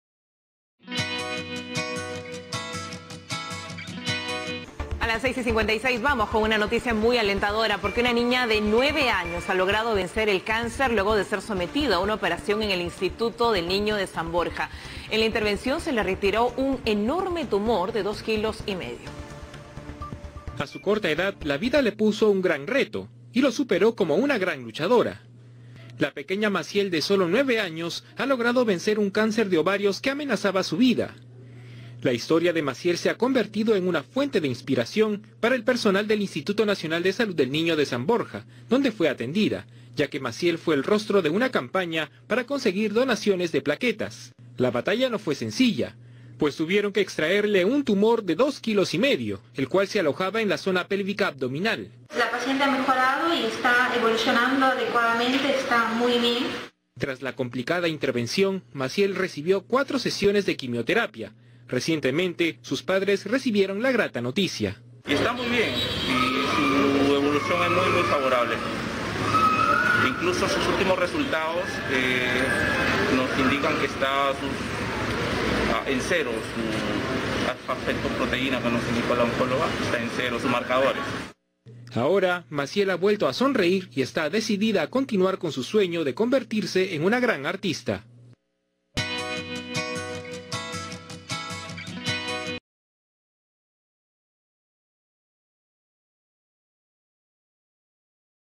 Retiran tumor de dos kilos a niña de nueve años
Una niña de nueve años ha logrado vencer el Cáncer luego de ser sometida a una operación en el Instituto Nacional de Salud del Niño de San Borja.